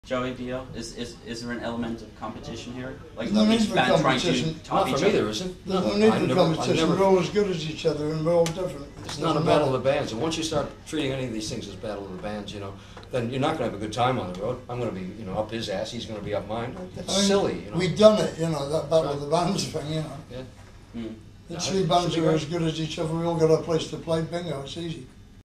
Rare Clip - Ronnie James Dio - Lemmy - Joey DeMaio interview (1999)
From our vaults:
Clip from an interview during the Monsters Of The Millenium Tour (1999) of DIO, MOTÖRHEAD & MANOWAR

The media was trying to pitch the bands against each other (sells better), while in reality, everybody had a great time together. In fact, we celebrated Thanksgiving together that year.

Sadly, Dio and Lemmy are gone. But they will forever be remembered for their amazing talent and for being incredible human beings - sincere, true, no bull!